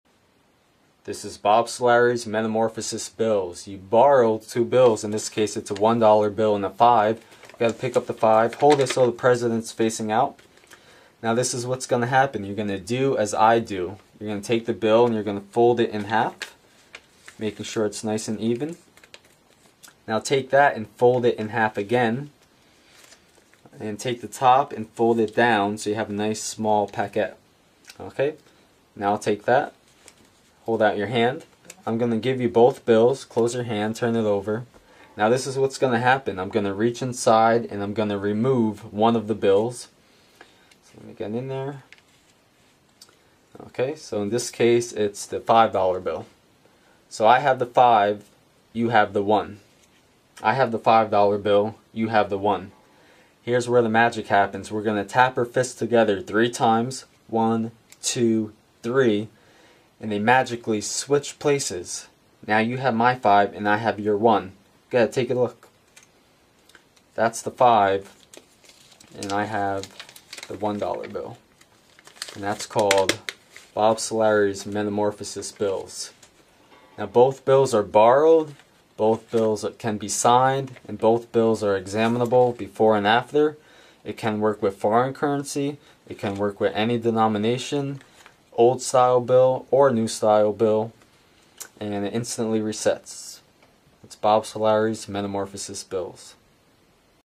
Metamorphosis Bill by Bob Solari - The Magic Attic
A One and Five Dollar Bill transpose under IMPOSSIBLE and FAIR conditions!

    Works with new style bills as well as old style
    Both bills can be borrowed
    Both bills can be signed
    No magnets or wax
    Any denomination bills can be used
    No sleight of hand
    Works with foreign currency as well

By far, this is the MOST incredible and SIMPLE bill transposition EVER conceived.

Complete with illustrated manuscript and sample functional gimmick.